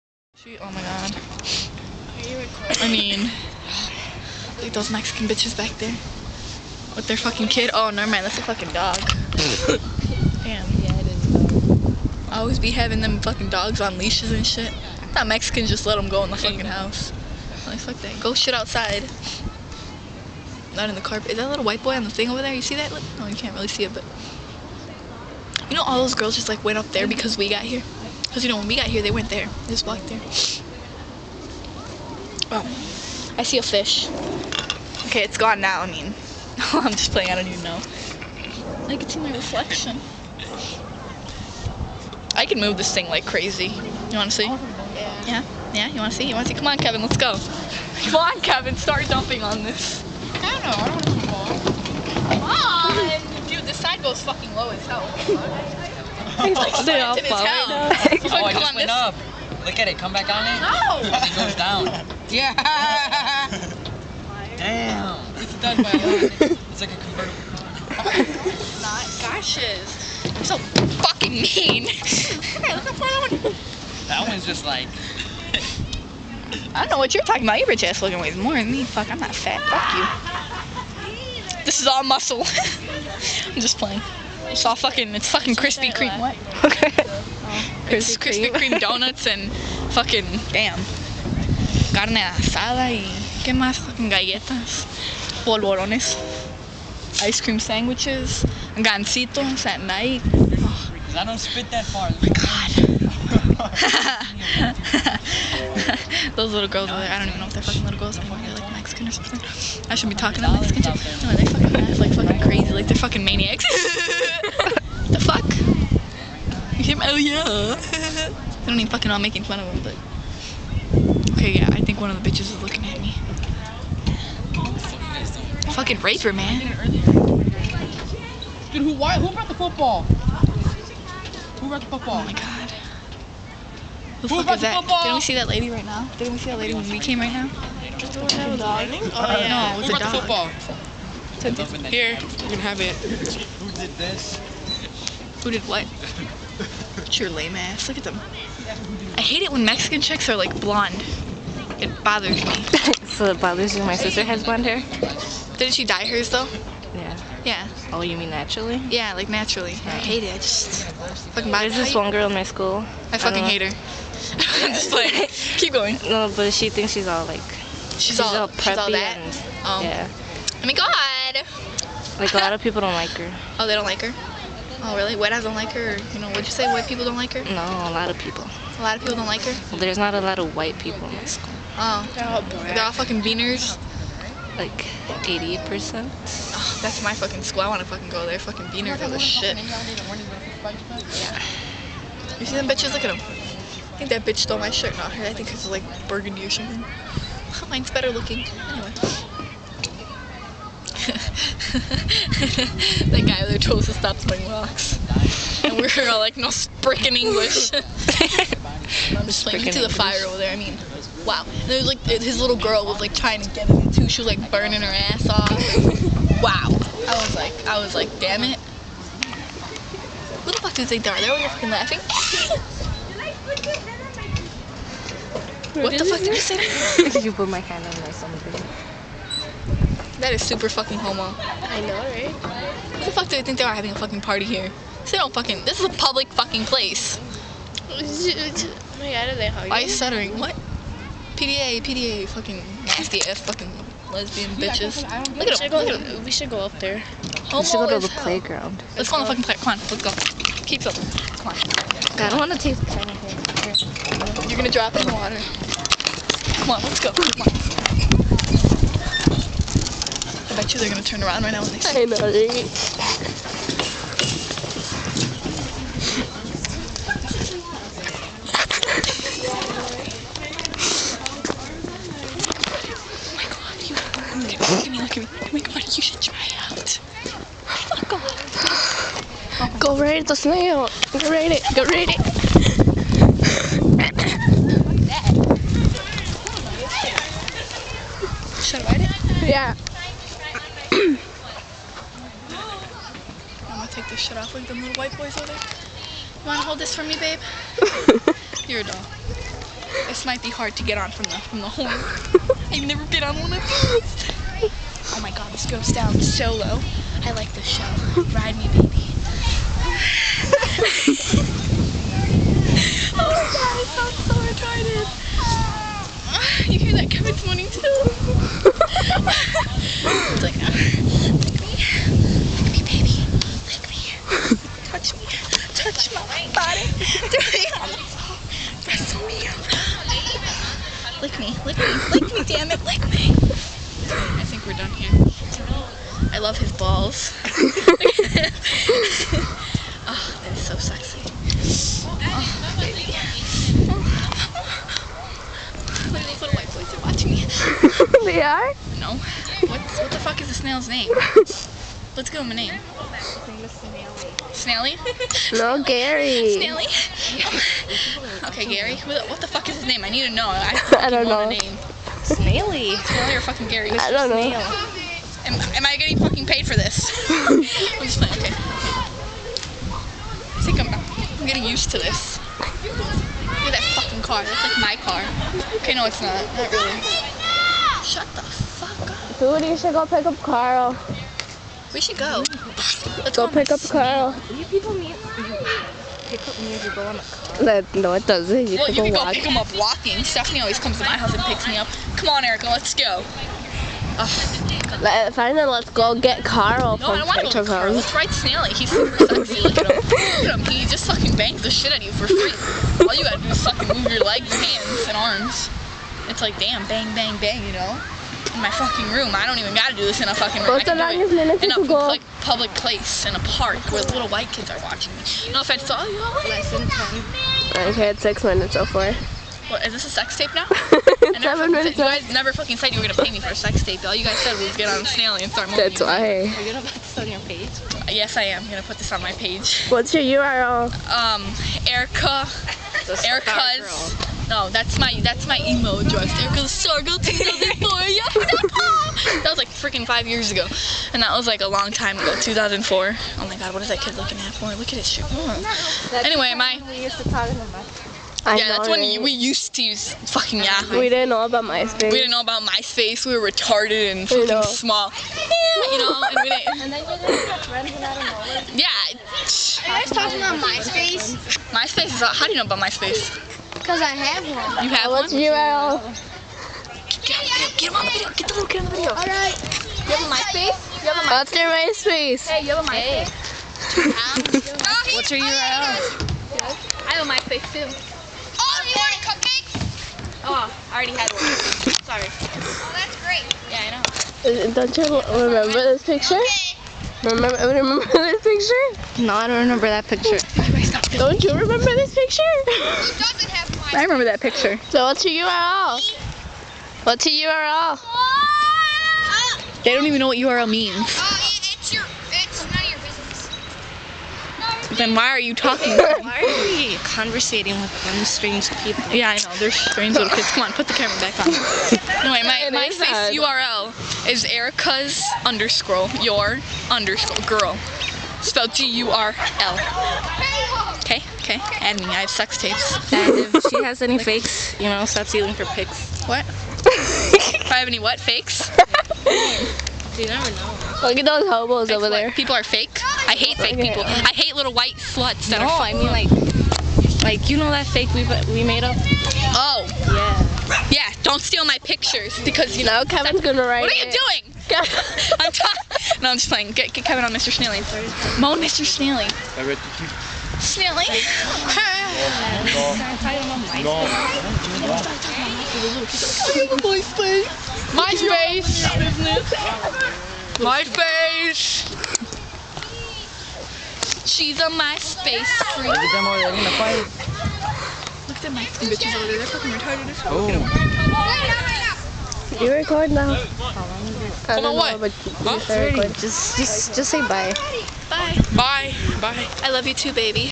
Erika's Video of how do you sleep...
erika talking about some girls at the little house thingy and then making a sort kind of sex tape with snaily the snail